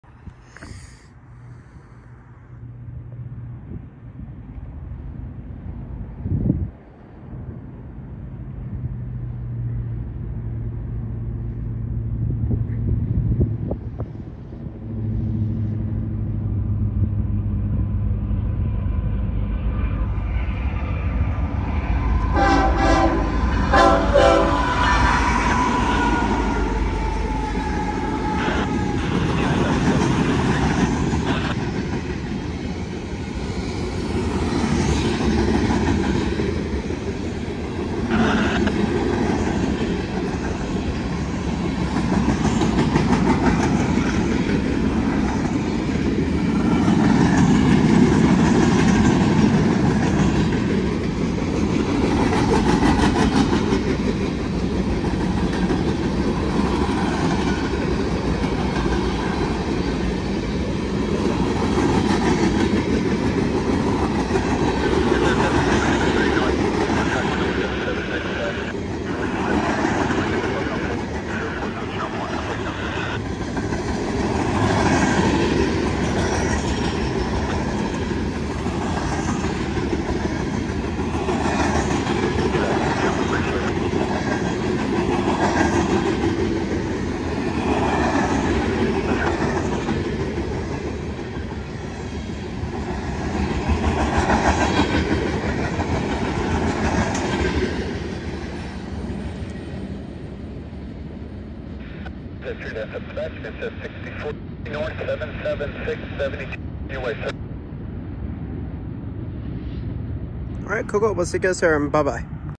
Union Pacific #7372 Leading & Short SB MNYPU With Three Power Unit’s 9/2/23.
So The Next Train Was SB Union Pacific MNYPU With Three Power Unit’s To Pueblo For This Short UP Manifest With No DP Unit’s & With Leading UP AC45CCTE Leading This Awesome Three Power Freight Train Down The Pike Peak Subdivision Hope You Enjoy This Beautiful Video.

Union Pacific SB MNYPU At 11:03pm
#7372 GE AC45CCTE
#8663 EMD SD70ACe
#4528 EMD SD70M